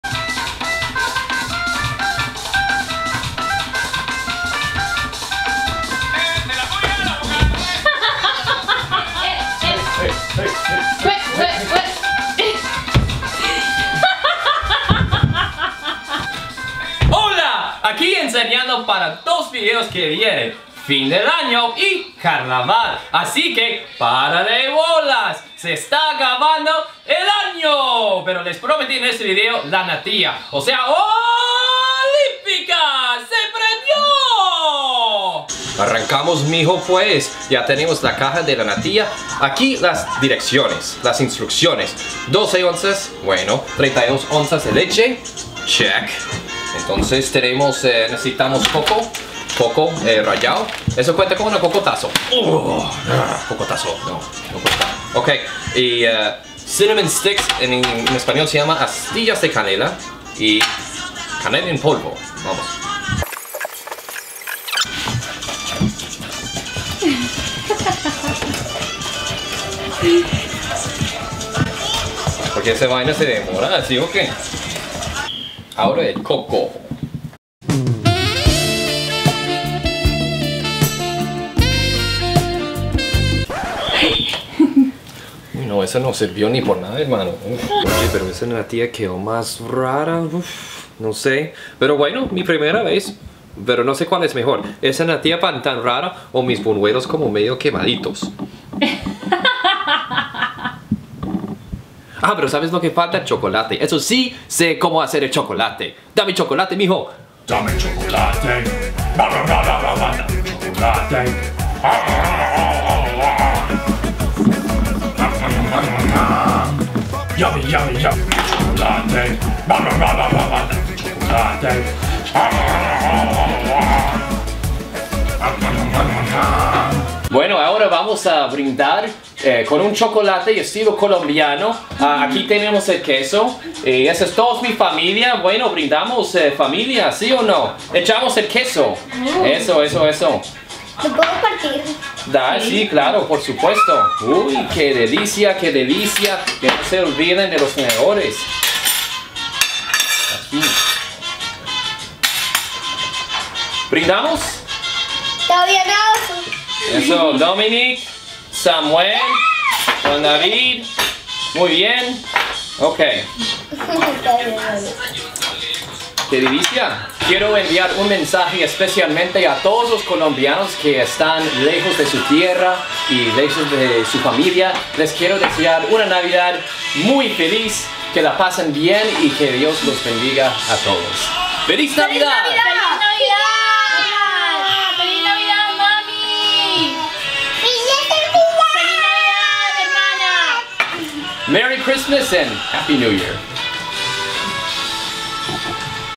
Gringo Prepara la Natilla Colombiana
Árbolito, natilla, buñuelos, párale bolas, ¡ya se acerca la Navidad! Aquí este gringo cumple la promesa de prepararles lo que queda en la lista de las delicias navideñas de Colombia.